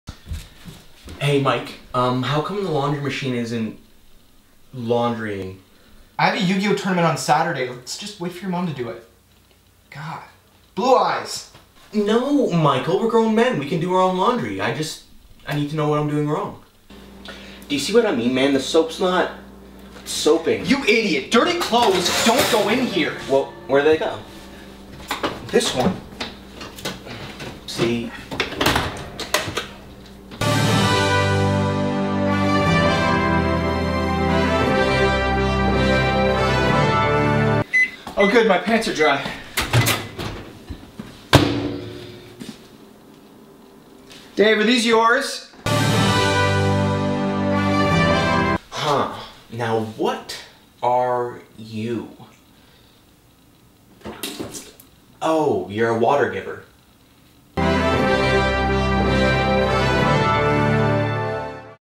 Laundry Time || Mike & Dave
Mike and Dave step up and do their own laundry for the first time. It doesn't go well. (Sketch Comedy)

For more comedy from RECENTLY ADULTS.